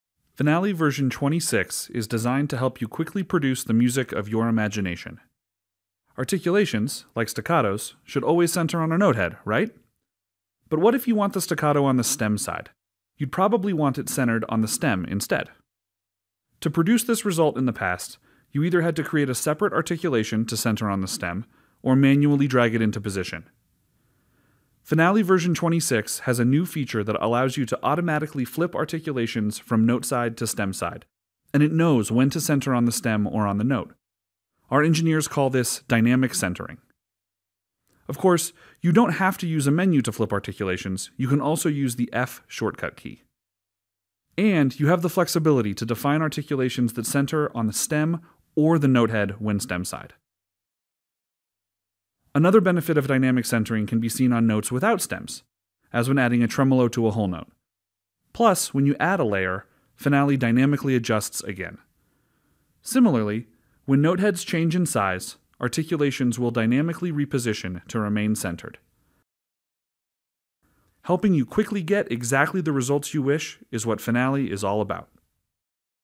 Dynamic Centering in Finale Version 26
See a sneak preview of how dynamic centering will save you time when using Finale v26 (due later in 2018).

Finale version 26 is designed to help you quickly produce the music of your imagination.

Articulations, like staccatos, should always center on a notehead, right?

But what if you want the staccato on the stem side? You’d probably want it centered on the stem, instead.

To produce this result in the past you either had to create a separate articulation to center on the stem, or manually drag it into position.

Finale version 26 has a new feature that allows you to automatically flip articulations from note side to stem side, and it knows when to center on the stem or the note. Our engineers call this “dynamic centering.”

Of course, you don’t have to use a menu to flip articulations, you can also use the F shortcut key.

And you have the flexibility to define articulations that center on stem OR notehead when stem side.

Another benefit of dynamic centering can be seen on notes without stems, as when adding a tremolo to a whole note...

...plus when you add a layer, Finale dynamically adjusts again.

Similarly, when noteheads change in size, articulations will dynamically reposition to remain centered.

Helping you quickly get exactly the results you wish is what Finale is all about.